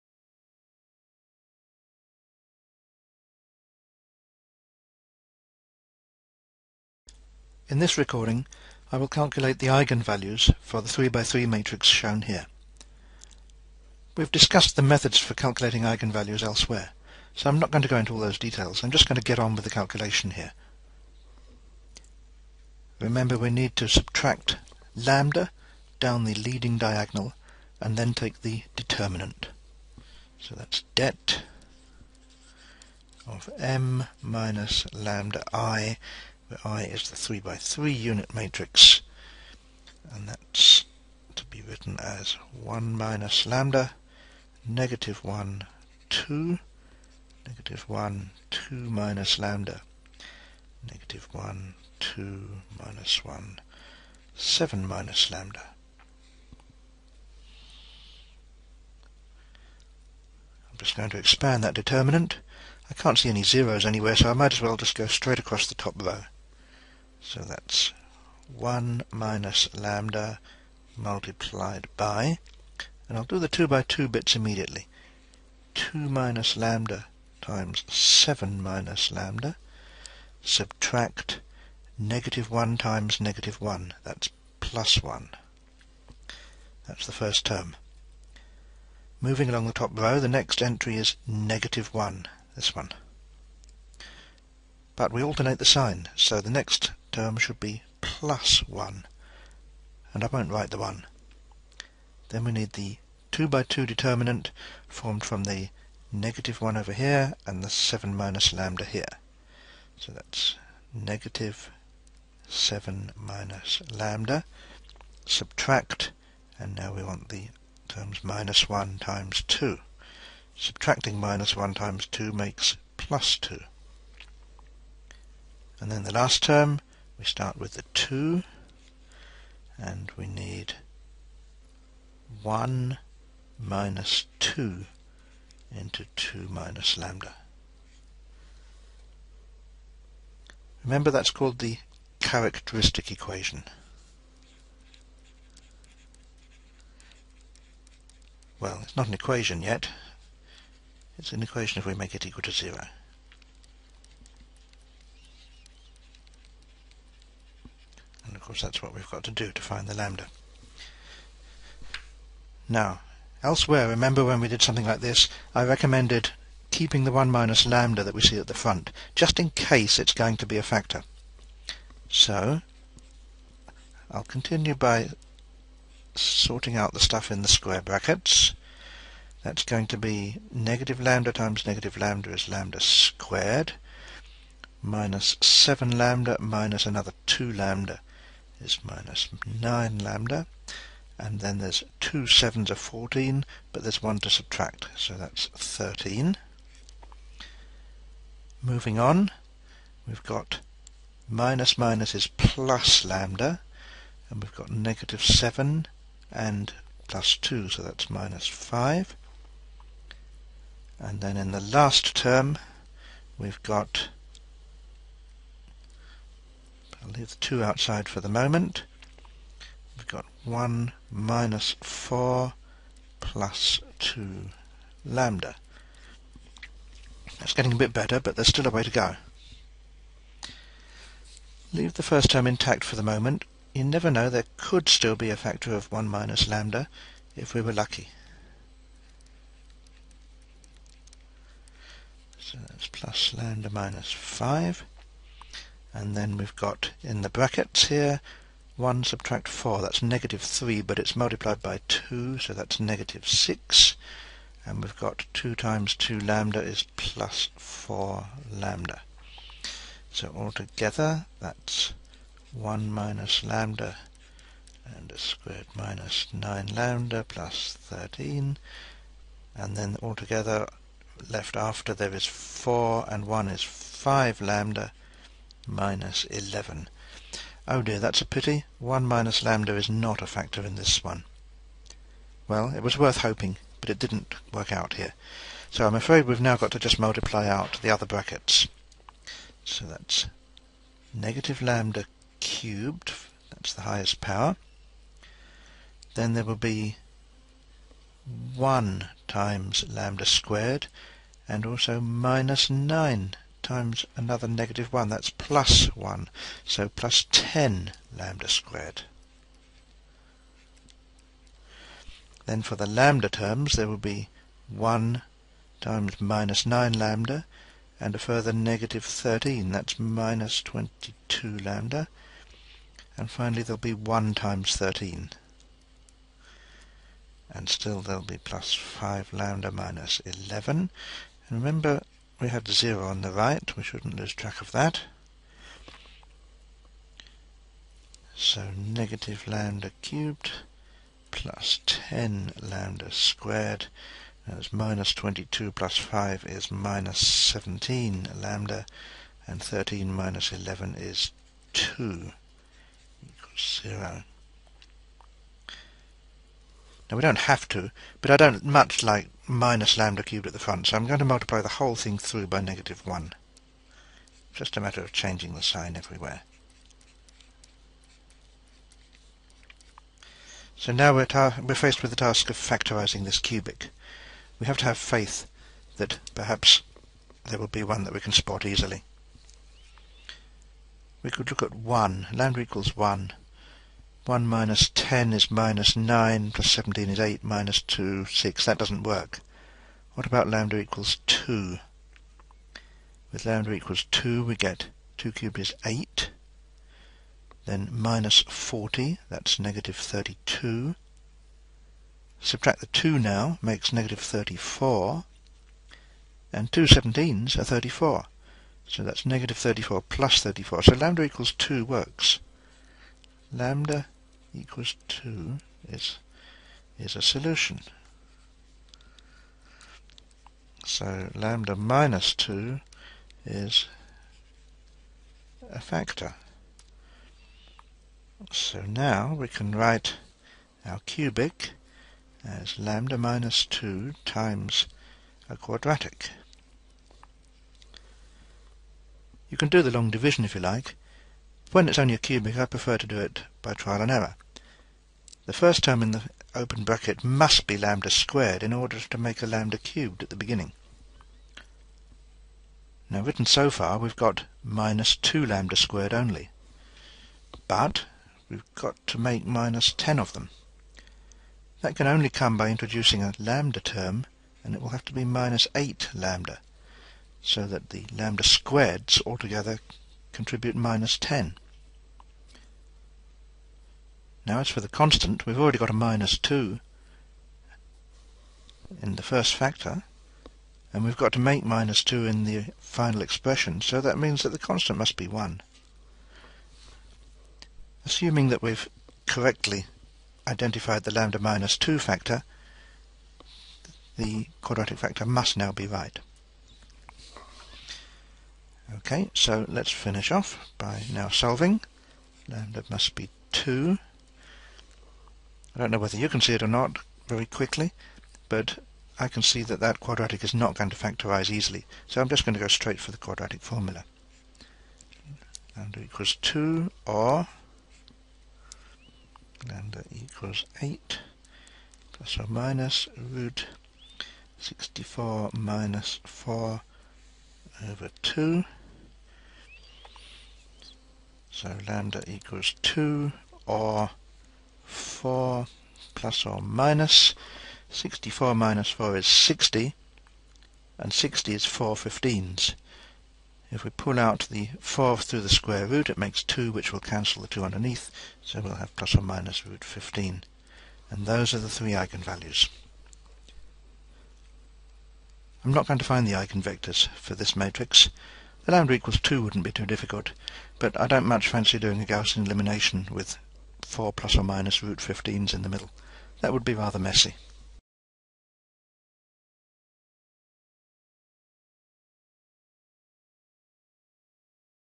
Another 3x3 eigenvalue example (MathsCasts)
We calculate eigenvalues of a 3 by 3 matrix.